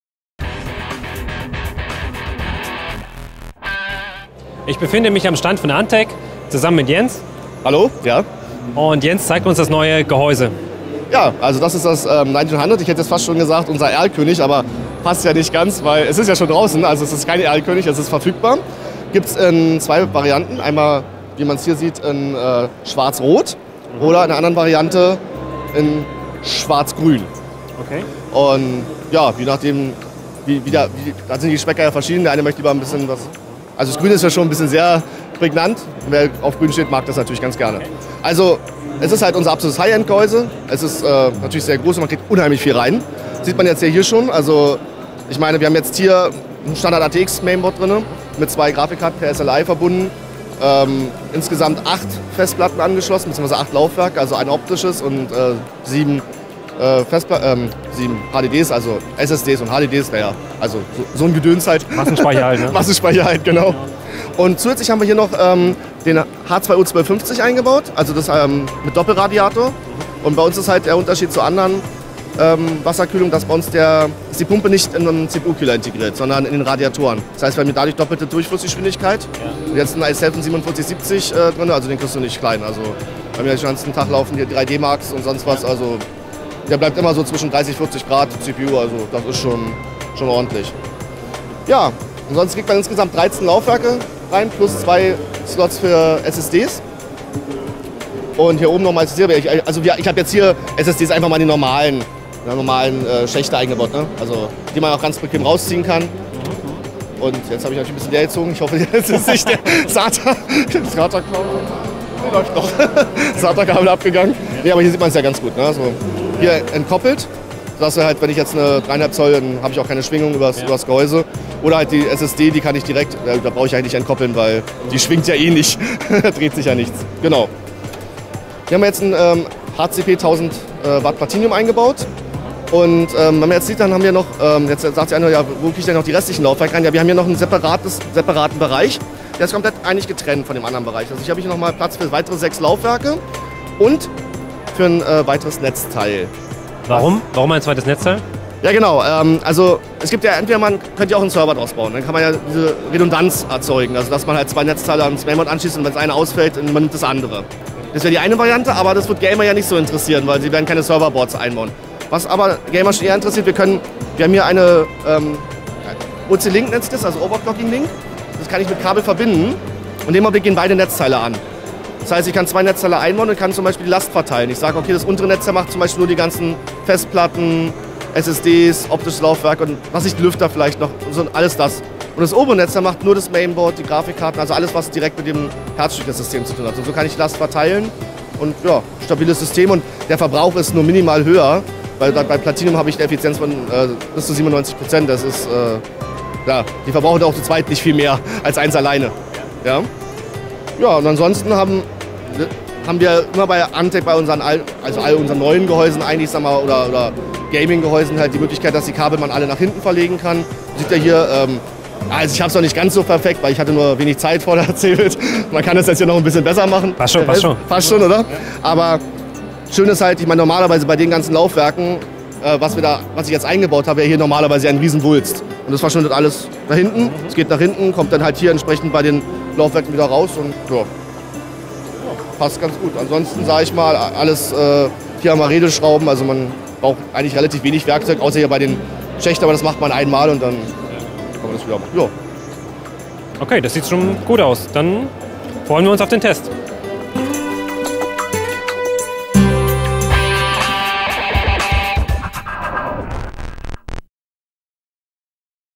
Antec | Big-Tower Nineteen Hundred vorgeführt | Cebit 2014
Auf der Cebit haben wir mit Jens Enders, Channel Sales Manager bei Antec, gesprochen, der uns unter anderem das Nineteen Hundred näher vorgestellt hat. Der Big-Tower eigne sich neben klassischen Desktop-Rechnern auch für Server- respektive Workstation-Systeme, bei denen eine Vielzahl an Festplatten und womöglich auch zwei Netzteile genutzt werden sollen.